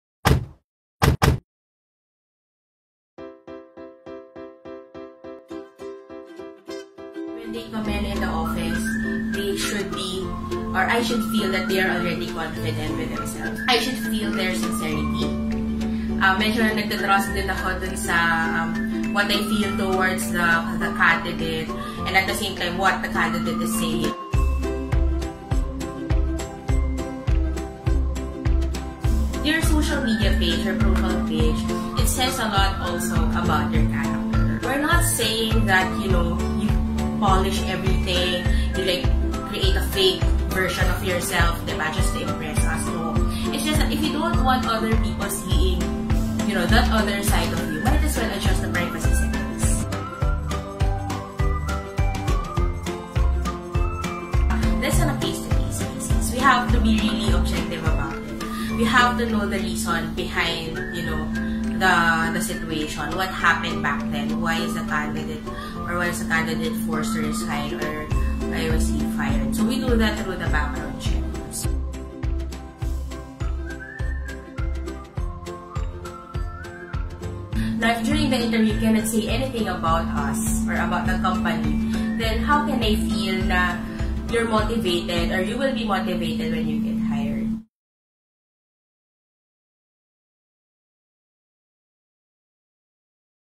Get Hired! Tips Para Matanggap sa Trabaho
How should applicants present themselves during job interviews? Do HR managers check the social media profiles of the applicants? Find out in this episode with Ayala Land HR Associate Manager Leah Yvette Alay-ay!